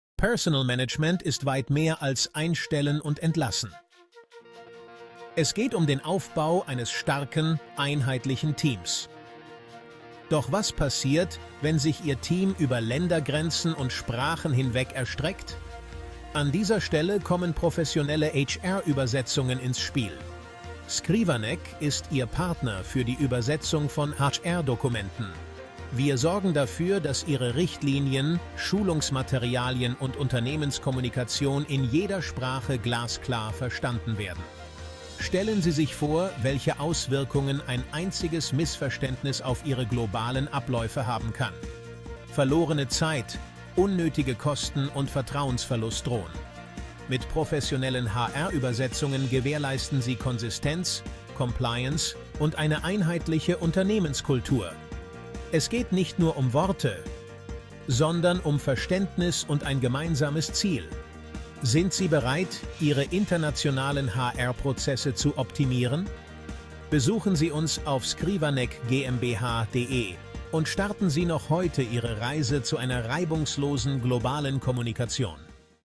Die Bedeutung von HR Übersetzungen für Ihr internationales Unternehmen
Erfahren Sie mehr über die wahre Bedeutung von Personalmanagement und wie es weit über das einfache Einstellen und Entlassen hinausgeht. Wir tauchen tief in die Rolle professioneller HR-Übersetzungen ein und zeigen, wie sie helfen, Sprachbarrieren zu überwinden. Entdecken Sie, wie effektives HR-Management durch präzise Übersetzungen die Kommunikation verbessert und so eine produktive und inklusive Arbeitsumgebung schafft.

Wenn Ihnen dieses Video gefällt, vergessen Sie bitte nicht, es zu liken und zu teilen, um anderen zu helfen, wertvolle Einblicke in modernes Personalmanagement zu gewinnen!